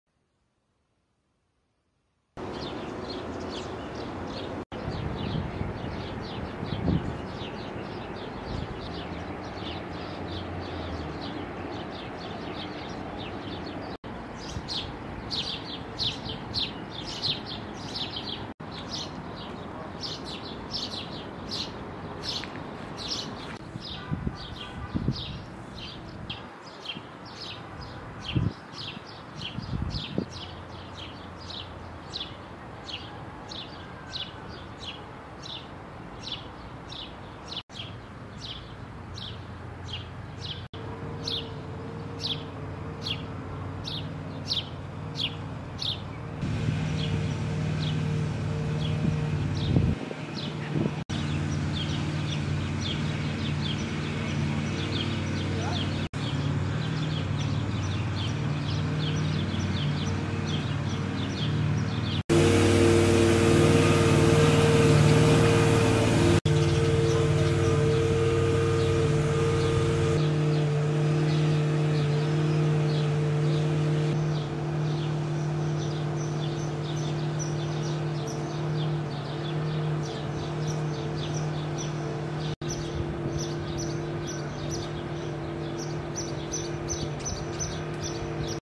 BIRCA BELEK'TE BİTMEYEN RESTORASYON
Birca Belek’te Şubat 2023 tarihine kadar bitirilmesi kararlaştırılan restorasyon çalışmaları, Ekim 2024’e kadar uzatıldı.